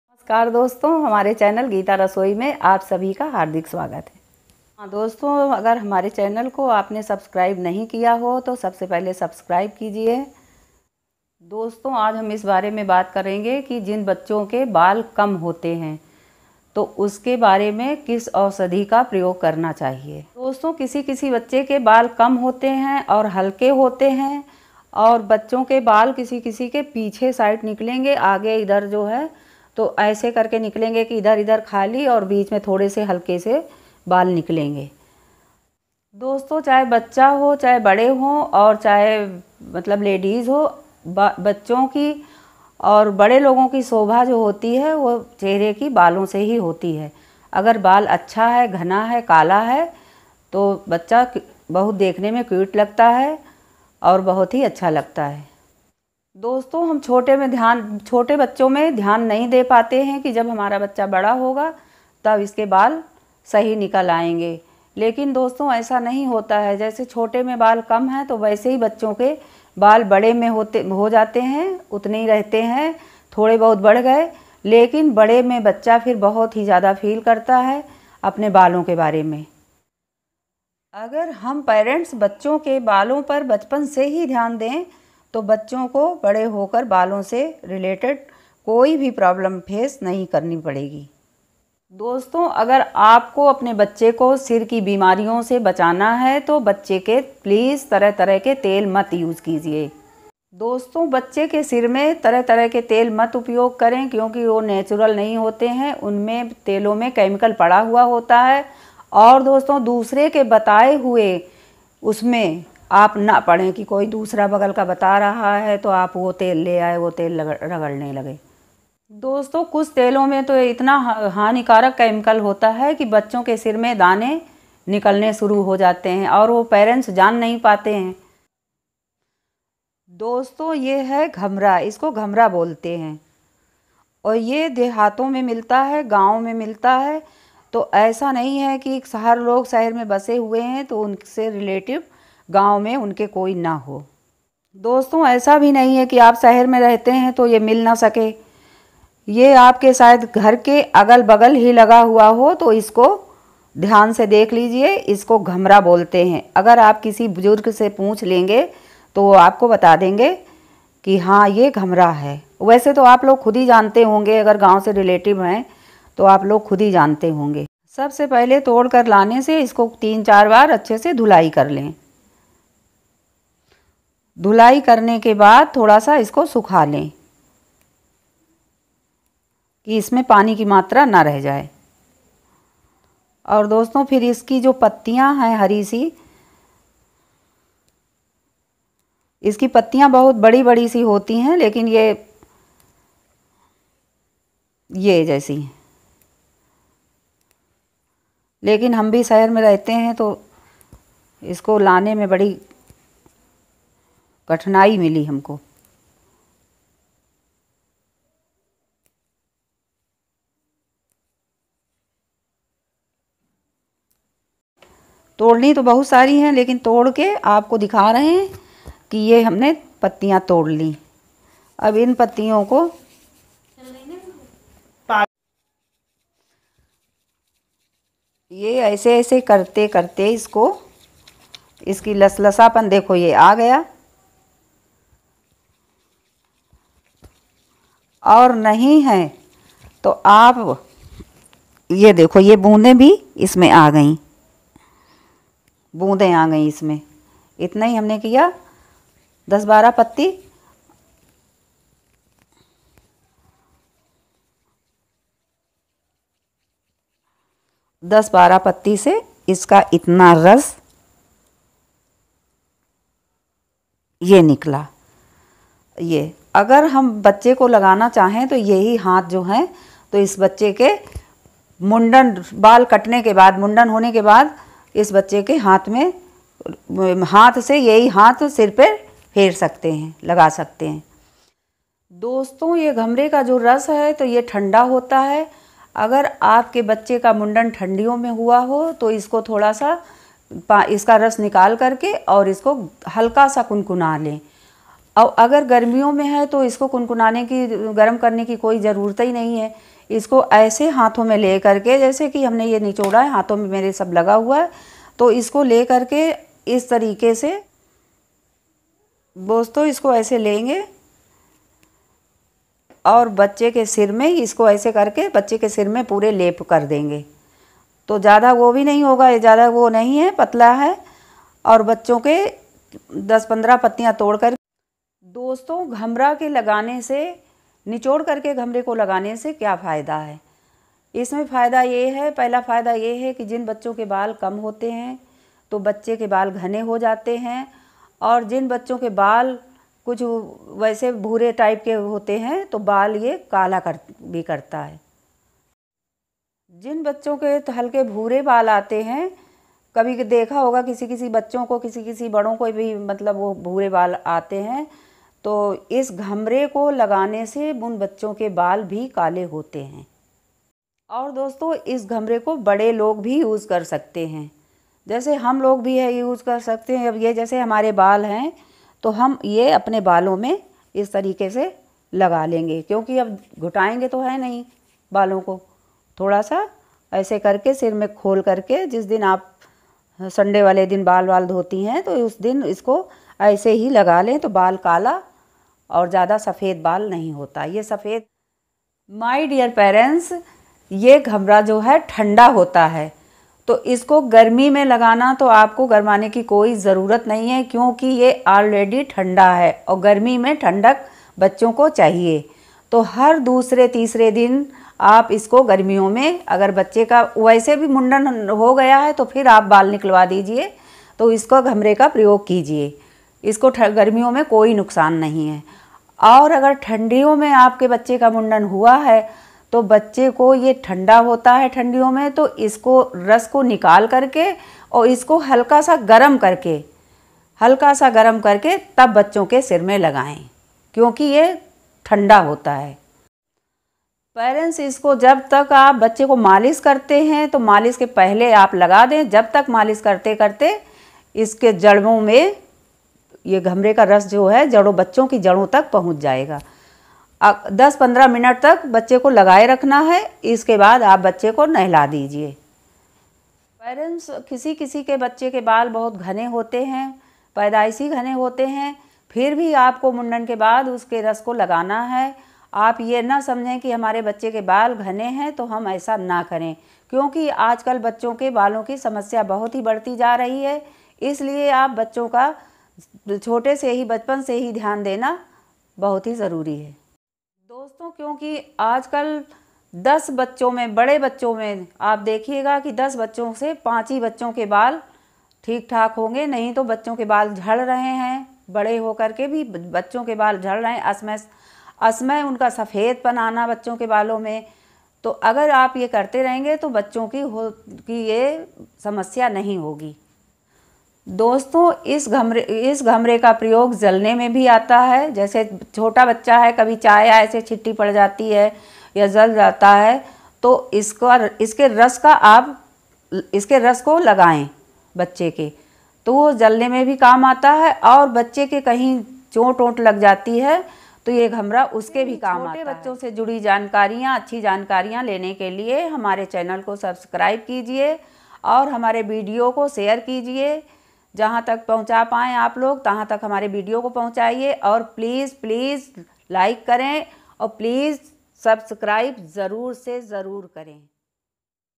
छोटे बच्चों के सर में बाल नहीं निकल रहे हैंl आधे सर में निकल रहे हैं आधे में नहीं ! समस्या
छोटे बच्चों के सर में बाल नहीं निकल रहे हैंl आधे सर में निकल रहे हैं आधी में नहीं ! वीडियो देखें और जाने